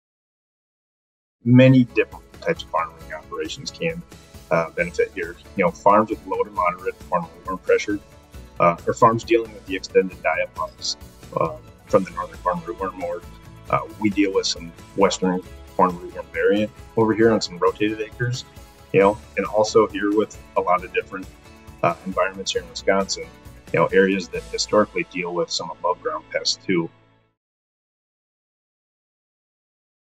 Cab Conversations | DEKALB® Brand With VT4PRO™ With RNAi Technology | How You Know
What’s the outlook on how DEKALB® VT4PRO™ products may be performing? Illinois farmer Pierce Bachman is very optimistic.